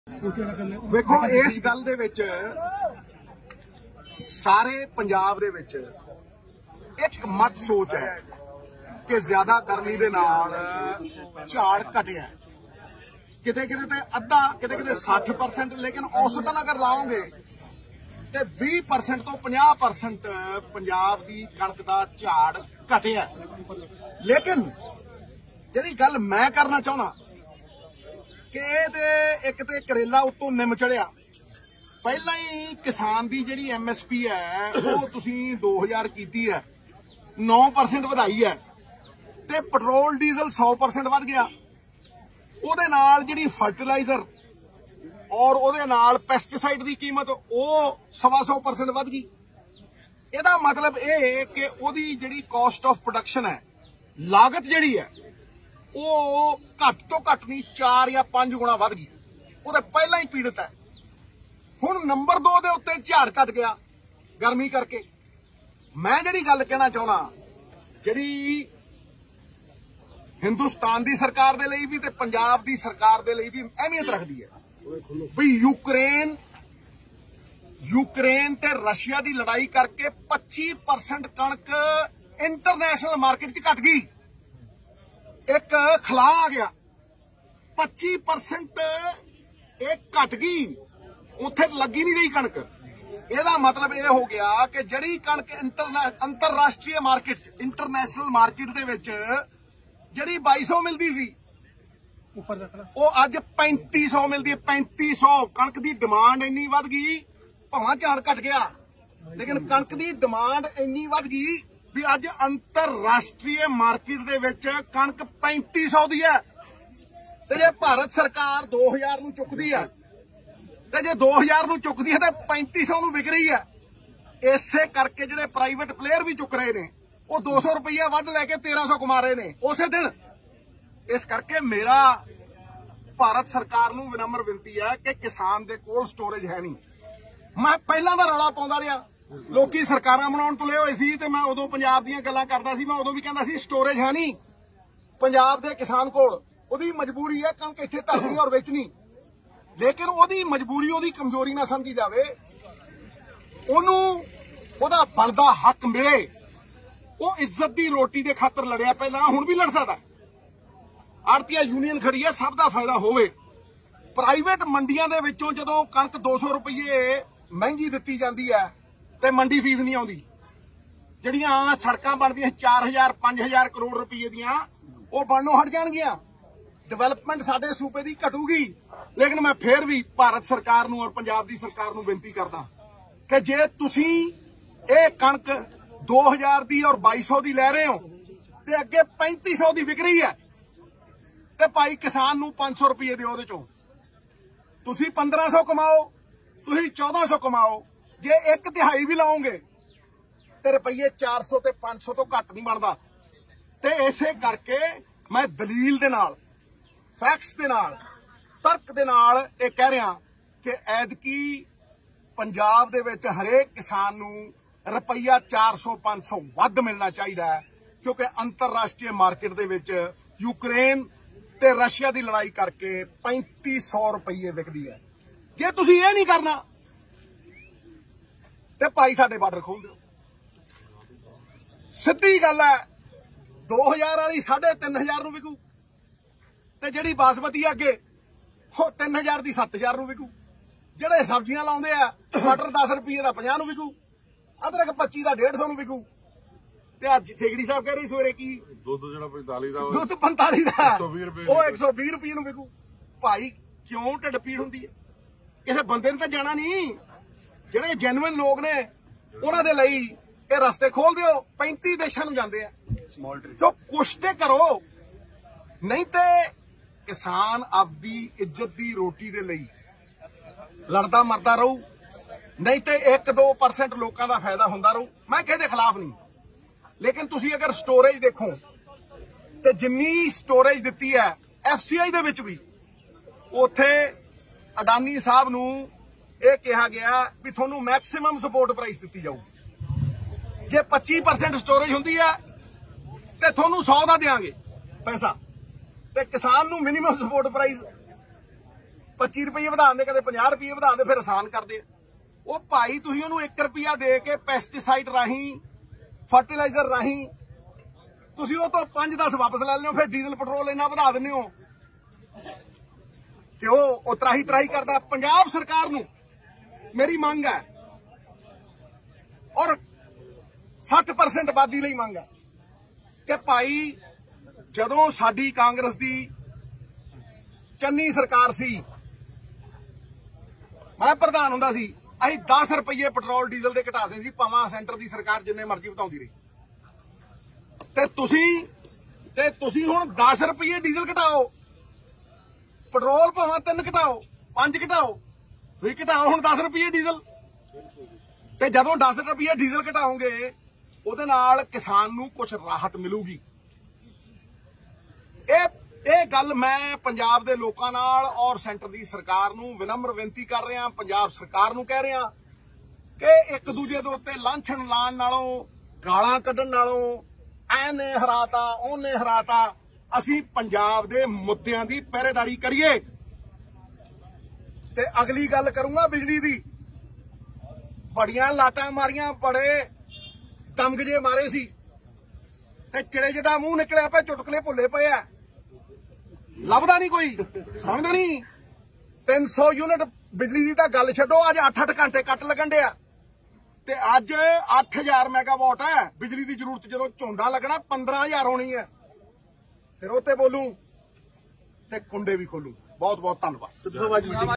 ਜਦੋਂ ਚੰਨੀ ਸਰਕਾਰ ਸੀ ਮੈਂ ਪ੍ਰਧਾਨ ਸੀ ਪੈਟਰੌਲ-ਡੀਜ਼ਲ ਦੇ ਰੇਟ 10 ਰੁਪਏ ਘਟਾ ਦਿੱਤੇ ਸੀ : Navjot Singh Sidhu
Live Tej Channel Presents : ਜਦੋਂ ਚੰਨੀ ਸਰਕਾਰ ਸੀ ਮੈਂ ਪ੍ਰਧਾਨ ਸੀ ਪੈਟਰੌਲ-ਡੀਜ਼ਲ ਦੇ ਰੇਟ 10 ਰੁਪਏ ਘਟਾ ਦਿੱਤੇ ਸੀ, AAP ਵਾਲਿਓਂ ਤੁਸੀਂ ਵੀ ਹੁਣ ਰੇਟ ਘਟਾਓ
ਮੈਂ ਭਾਰਤ ਅਤੇ ਪੰਜਾਬ ਸਰਕਾਰ ਨੂੰ ਬੇਨਤੀ ਕਰਦਾ ਹਾਂ ਕਿ ਕਿਸਾਨਾਂ ਨੂੰ ਐਲਾਨੀ ਐਮ.ਐਸ.ਪੀ. ਤੋਂ ਉੱਪਰ ਘੱਟੋ-ਘੱਟ 400 ₹ ਪ੍ਰਤੀ ਕੁਇੰਟਲ ਮੁਆਵਜ਼ਾ ਦਿੱਤਾ ਜਾਵੇ ਨਵਜੋਤ ਸਿੱਧੂ  || Live Tej Channel